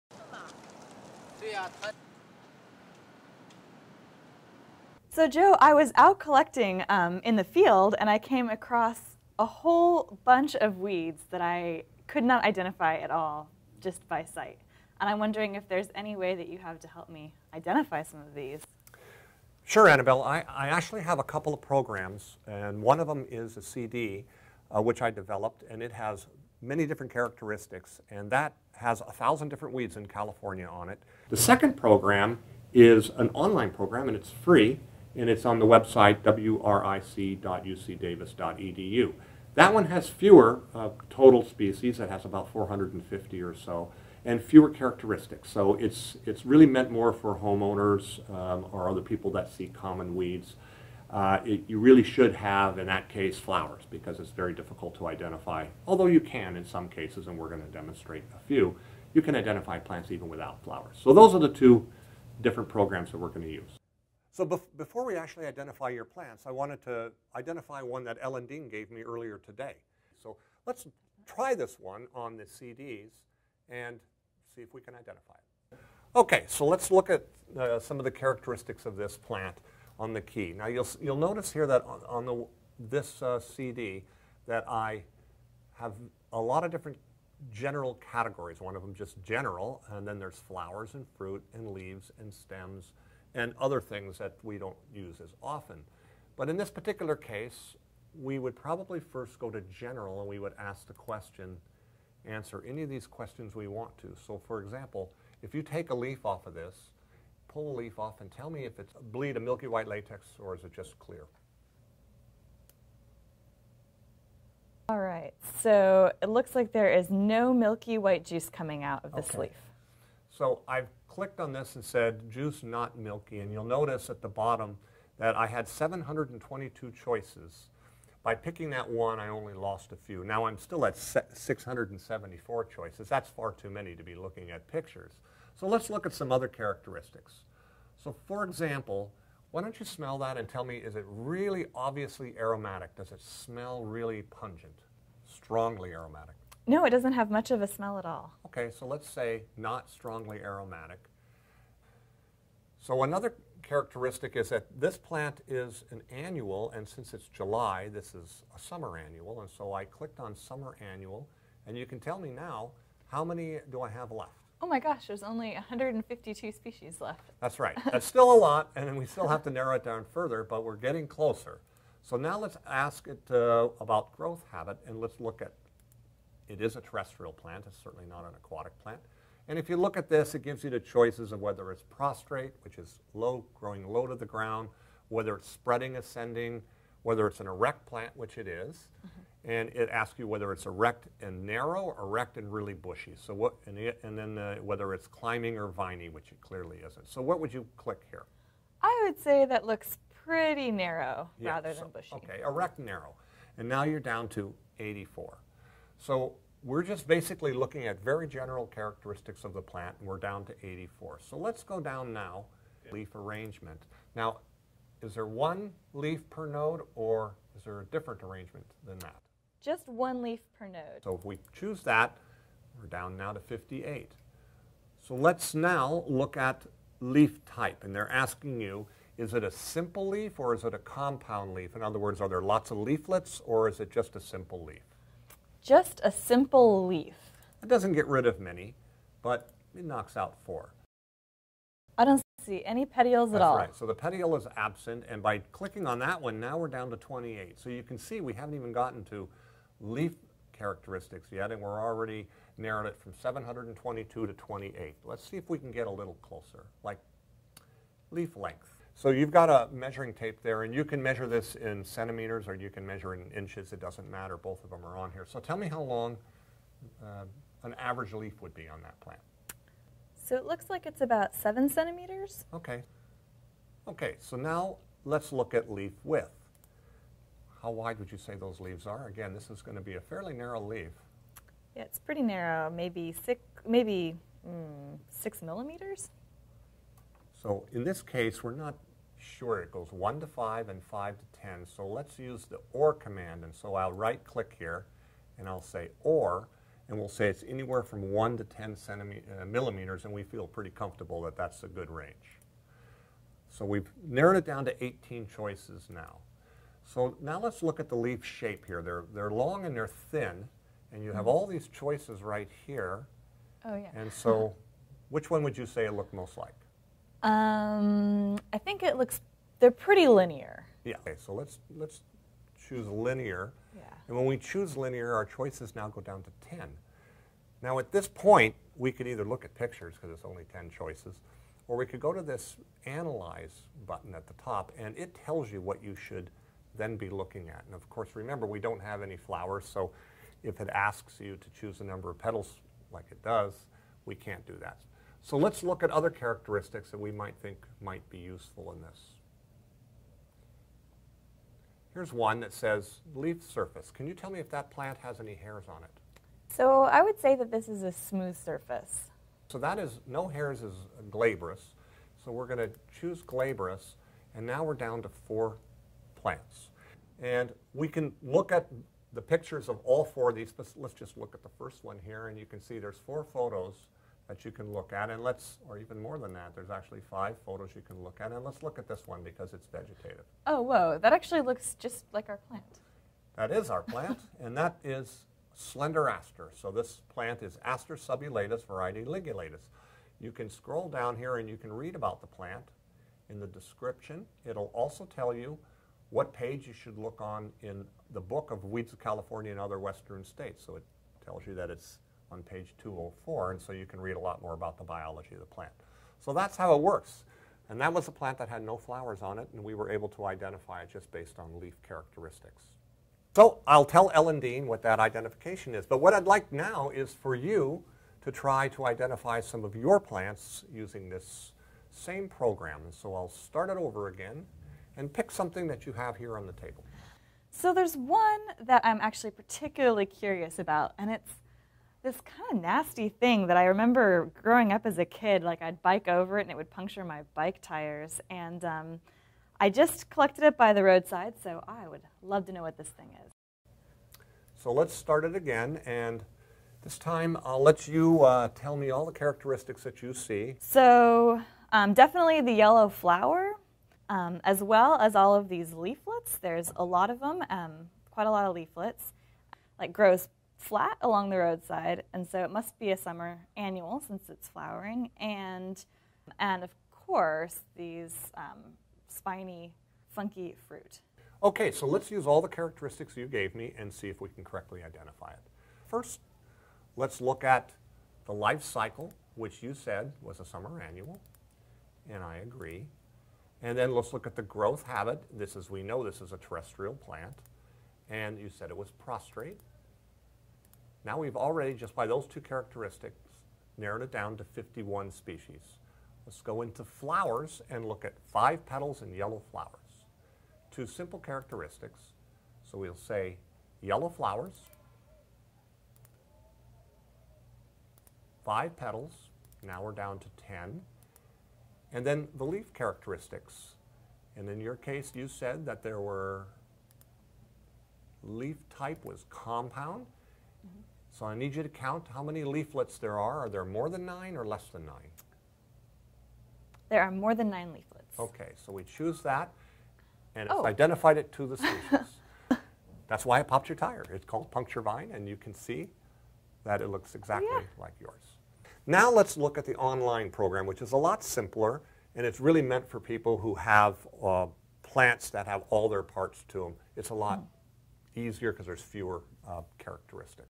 Weed Identification - Part 2
Joe Ditomaso, of U.C. Davis, takes us an a tour of how to identify common weeds using traditional methods employed in professional settings like the U.C. Davis herbarium and also the latest and easiest techniques that employ the use of sophisticated computer programs available to the public.